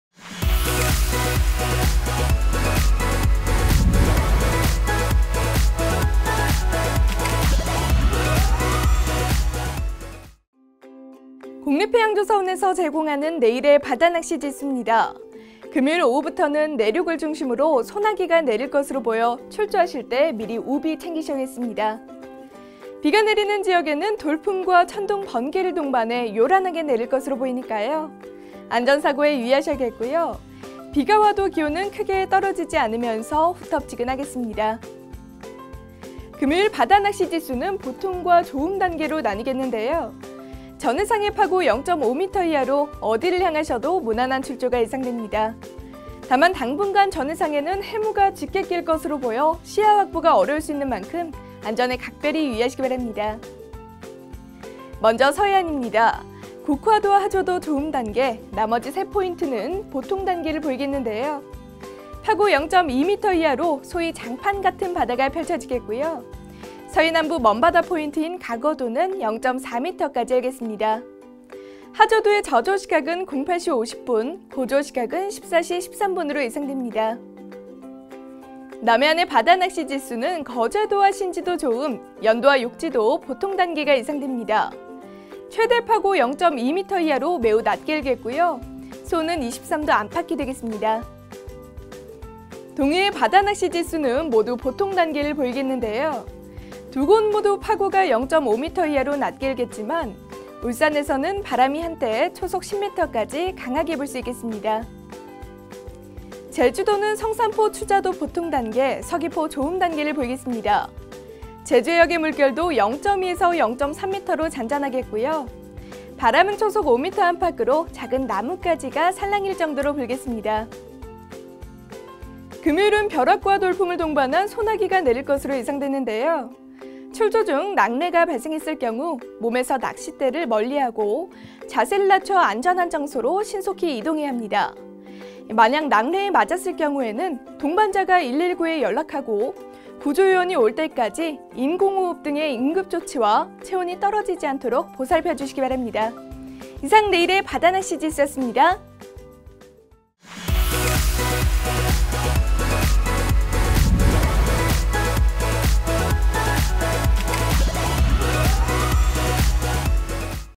[바다낚시지수] 7월14일 오후부터 내륙을 중심으로 소나기, 우비 챙기세요 /해양방송 On바다
▶ 기사 원문 :
국립해양조사원에서 전하는 내일의 바다낚시지수입니다.
금요일 오후부터, 내륙을 중심으로 소나기가 내릴 것으로 보여
출조하실 때 미리 우비 챙기셔야겠습니다.
비가 내리는 지역에는 돌풍과 천둥 번개를 동반해
요란하게 내릴 것으로 보이니까요. 안전사고에 유의하셔야겠고요.
비가와도 기온은 크게 떨어지지 않으면서 후텁지근하겠습니다.

금요일 바다낚시지수는 보통과 좋음 단계를 보이겠습니다.
전 해상의 파고 0.5m이하로 어디를 향하셔도
무난한 출조가 예상됩니다.
다만 당분간 전 해상에는 해무가 짙게 낄 것으로 보여
시야 확보가 어려울 수 있는 만큼,
안전에 각별히 유의하시기 바랍니다.

먼저 서해안입니다.
국화도와 하조도 좋음단계, 나머지 세 포인트는 보통 단계를
보이겠습니다. 파고 0.2m 이하로 소위 장판 같은 바다가 펼쳐지겠고요.
서해 남부 먼바다 포인트인 가거도는 0.4m까지 일겠습니다.
하조도의 저조 시각은 08시 50분,
고조시각은 14시 13분으로 예상됩니다.

남해안의 바다낚시지수는 거제도와 신지도 좋음,
연도와 욕지도 보통단계가 예상됩니다.
최대파고 0.2m 이하로 매우 낮게 일겠고요.
수온은 23도 안팎이 되겠습니다.

동해의 바다낚시지수는 모두 보통 단계가 되겠는데요.
두 곳 모두 파고가 0.5m 이하로 낮게 일겠지만,
울산에선 바람이 한때 초속 10m까지 강하게 불 수 있겠습니다.

제주도는 성산포 추자도 보통, 서귀포 좋음단계를 보이겠습니다.
제주해역의 물결도 0.2에서 0.3m로 잔잔하겠고요.
바람은 초속 5m 안팎으로 작은 나뭇가지가 살랑일 정도로 불겠습니다.

금요일은 벼락과 돌풍을 동반한 소나기가 내릴 것으로 예상되는데요.
출조 중 낙뢰가 발생했을 경우, 몸에서 낚싯대를 멀리하고 자세를 낮춰
안전한 장소로 신속히 이동해야 합니다.
만약 낙뢰에 맞았을 때는 동반자가 119에 연락하고
구조 요원이 올 때까지 인공호흡 등의 응급조치와 체온이 떨어지지 않도록
보살펴 주시기 바랍니다.
이상 내일의 바다낚시 지수였습니다.